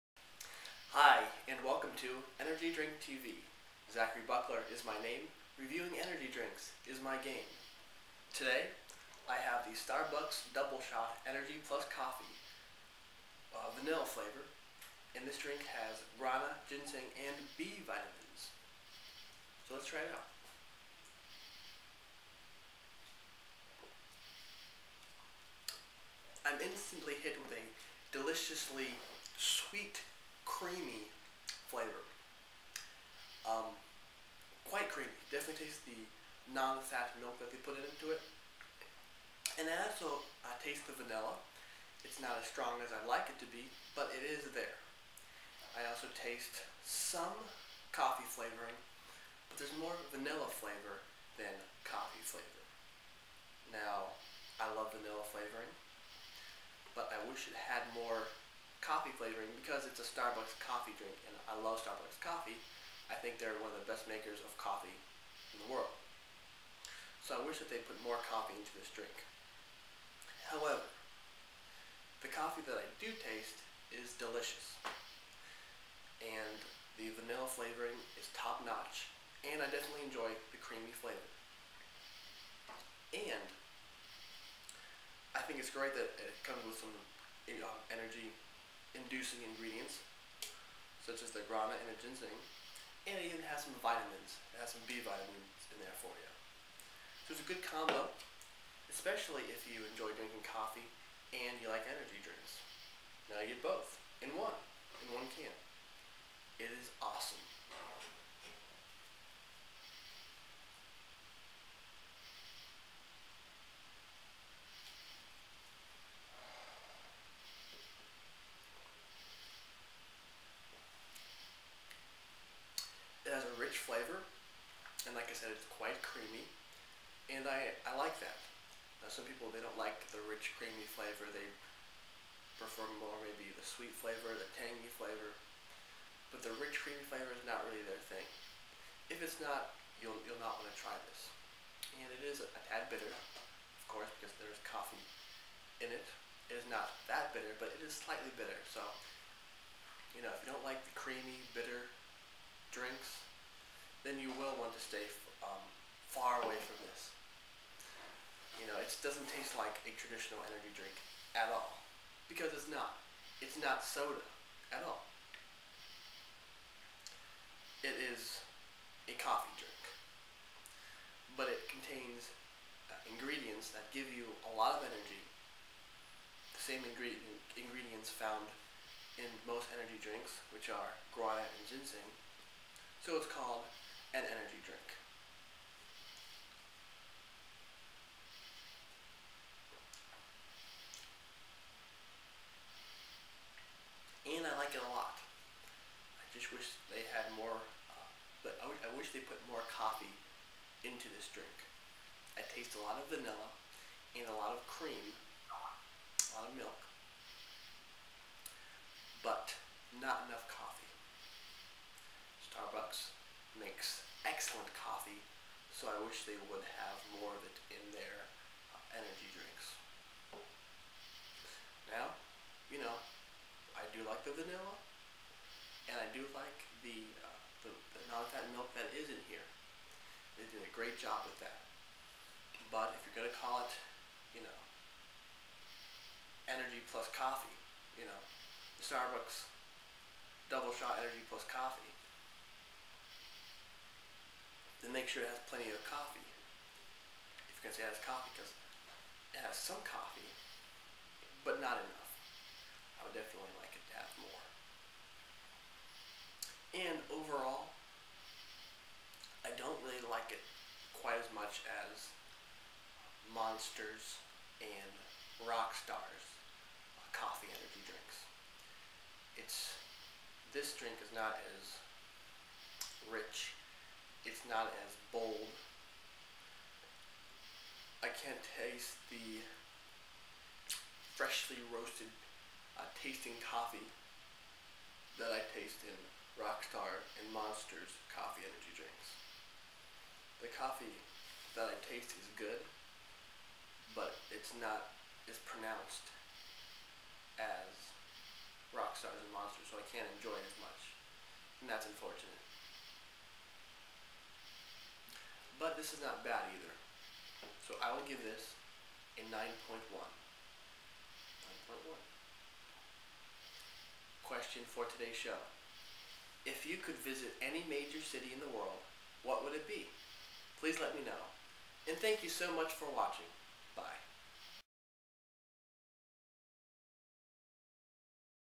Energy Drink TV #13
Today I review the Starbucks Energy+Coffee Vanilla energy drink